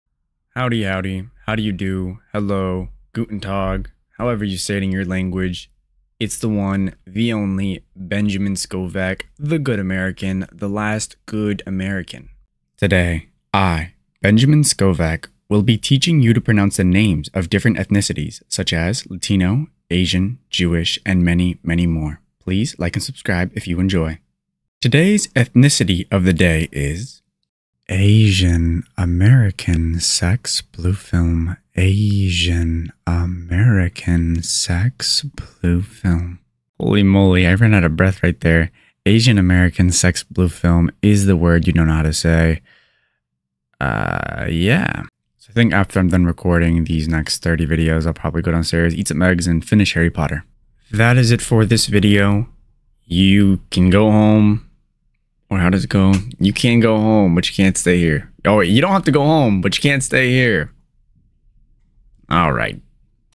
How to pronounce Asian American SEX BLUE FILM?(CORRRECTLY)
Welcome!
Throughout the entirety of my channel, you will learn to pronounce vocabulary the correct way. In today’s video, you will learn to pronounce Asian American SEX BLUE FILM the correct way.
Timestamps below:
0:00 intro
0:27 word of the day
0:37 examples of word
0:51 ending